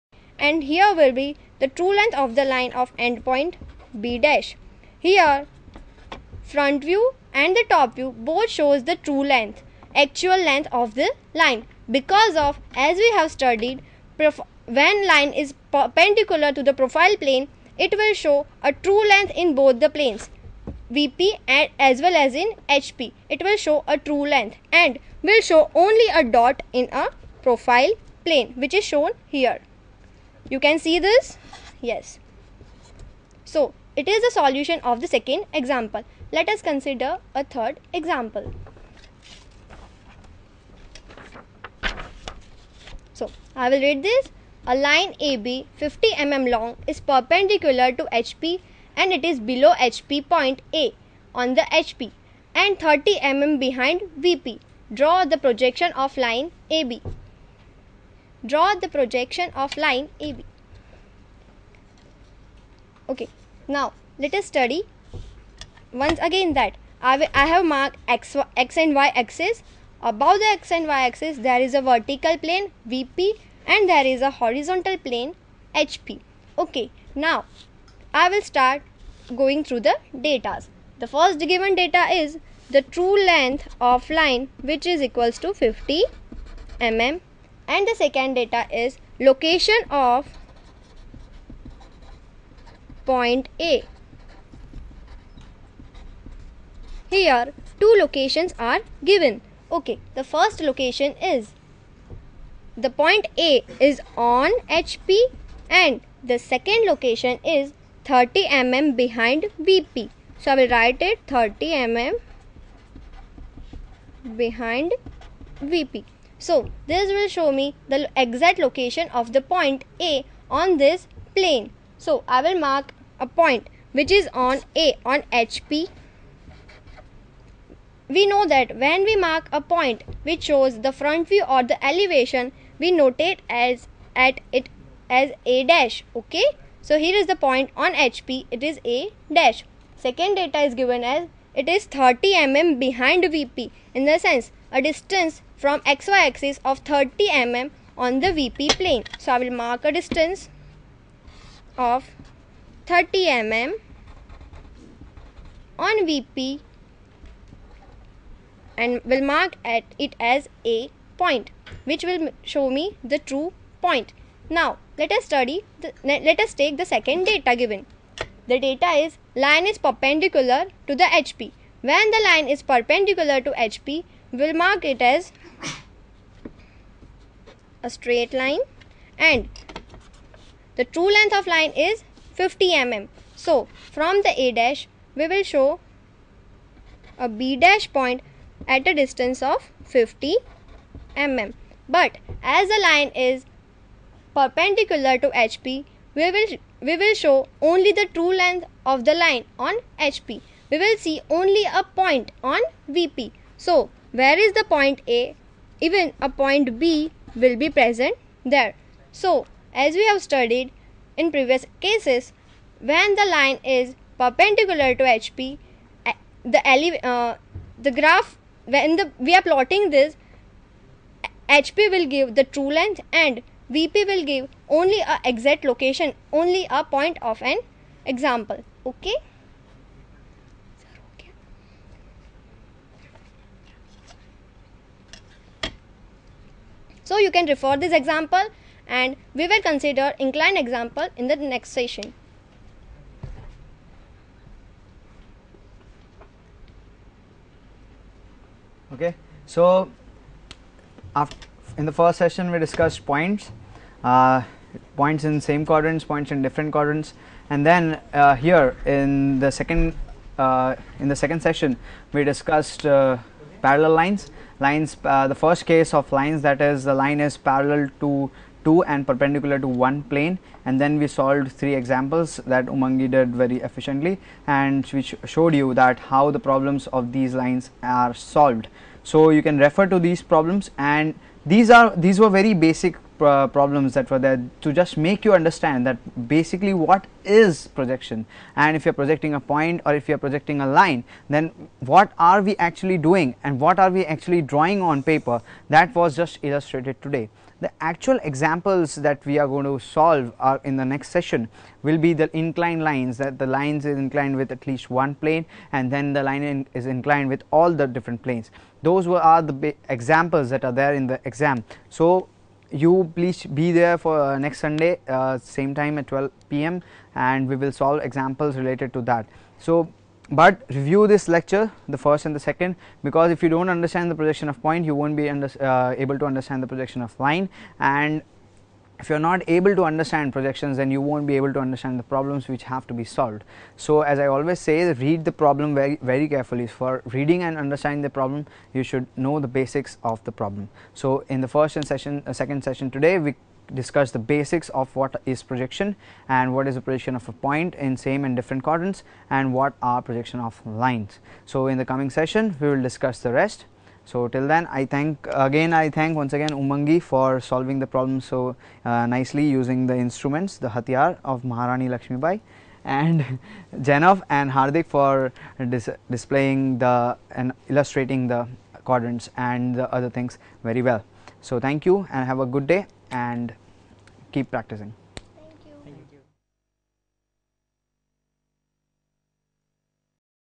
Yash Chawla Projections of Points & Lines 3
Yash Chawla-Parul Ins. Of Engg & Tech,Vadodara-Engineering Graphics-Projections of Points & Lines-Introduction to principal planes of projections, Projections of the points located in same quadrant and different quadrants, Projectin of line which is parallel to the plane.